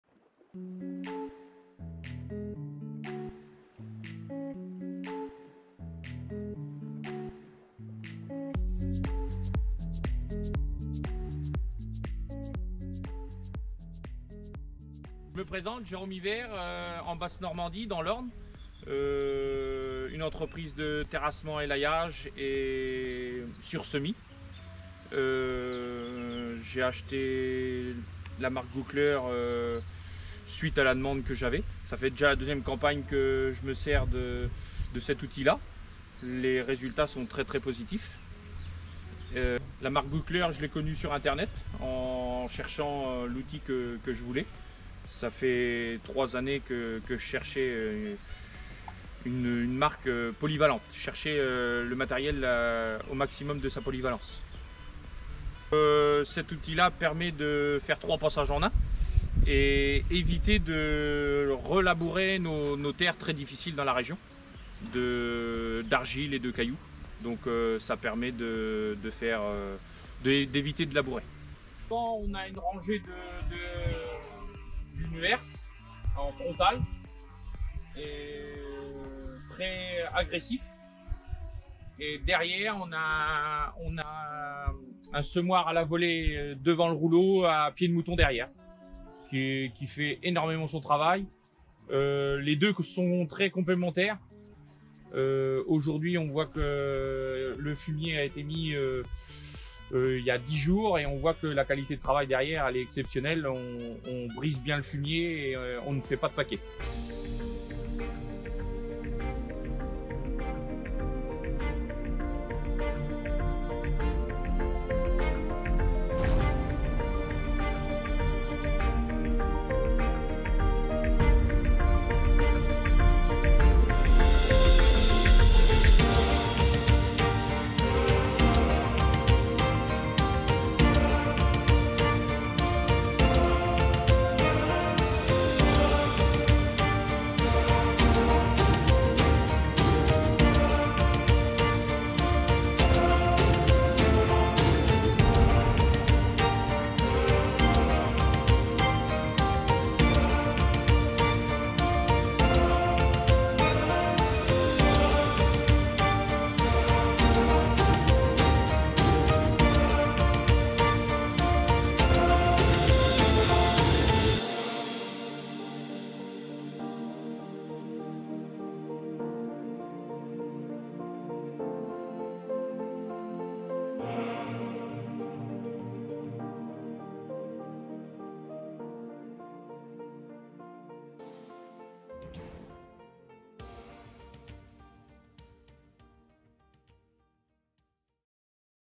GreenMaster 600 alpin GUTTLER sursemis prairie en Normandie - ETA Jerome Yver
Présentation du GreenMaster 600 Alpin par l'ETA Jérome Yver
Matériel de sursemis des prairies, regarnir sans labourer :
- équipé de doigts pour scarifier
- barre niveleuse pour étendre les taupinières et réparer dégâts de gibier
- herse étrille pour aérer le sol
- semoir pneumatique à la volée, semis sur toute le largeur. semoir simple et précis
- rouleau Matador 610 Guttler pieds de moutons pour raffermir et plomber le semis

aussi pour les nouveaux semis, semis d'engrais verts, etc.